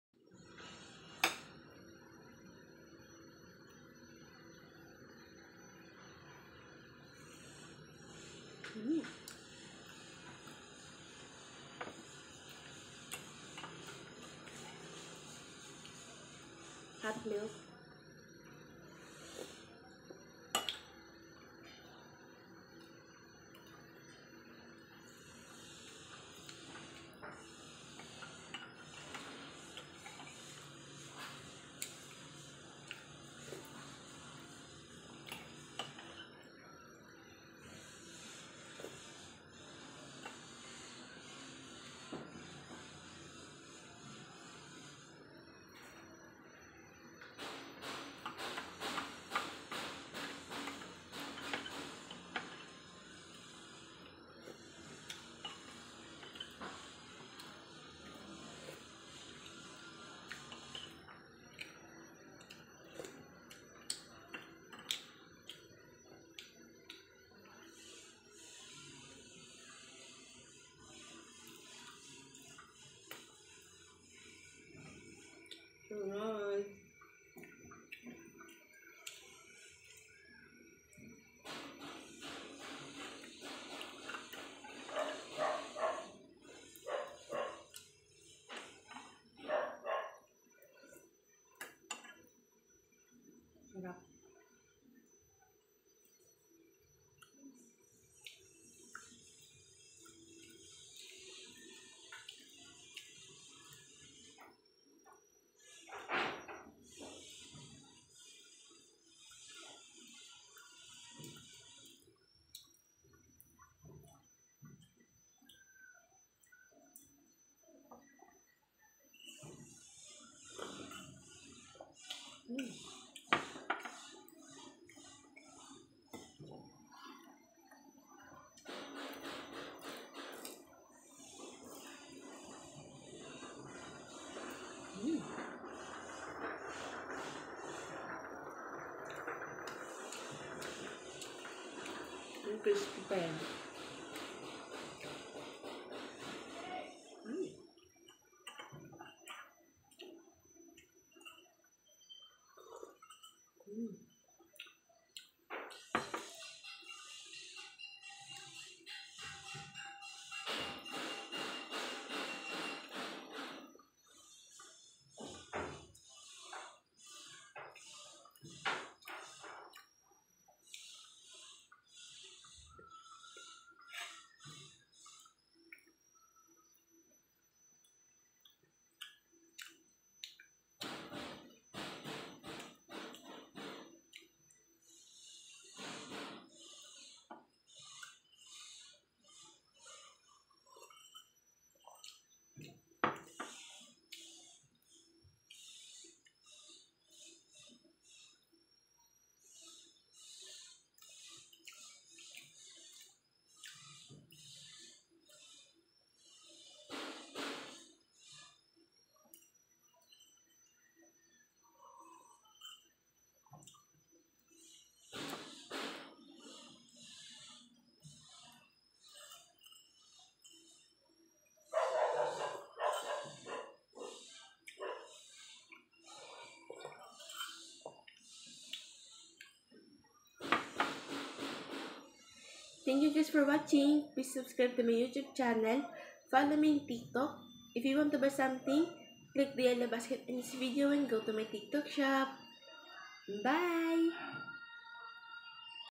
TURON BANANA WITH HOT MILK MUKBANG BREAKFAST MY DAILY ROUTINE
I just want to share my daily routine and my mukbang, my food for everyday and my unboxing especially to my YouTube channel. please subscribe to my YouTube channel, don't forget to like, share and comment. God bless all.